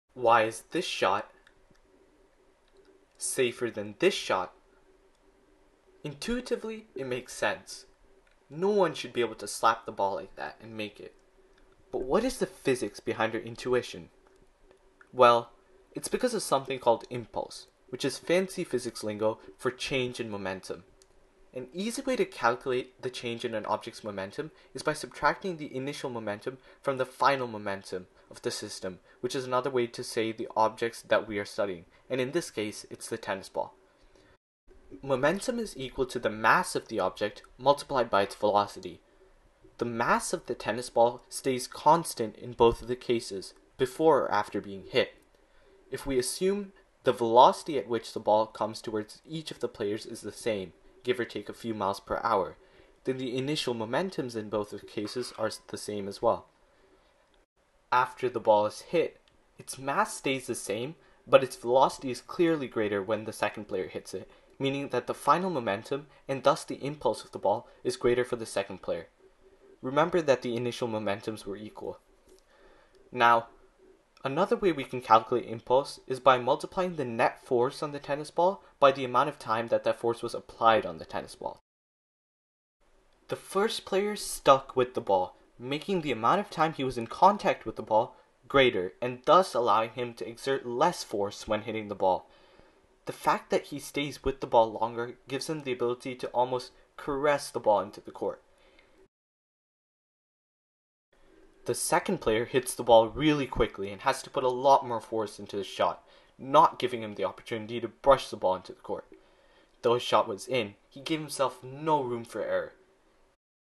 Why should you follow through/stick with your shot in tennis? | Impulse in sports
Why should you not slap the ball in tennis? Well, it has to do with something called impulse, or the change in an object's momentum. This video will explain this correlation in detail.

Credit for the clips goes to Tennis Corner and Tennis TV.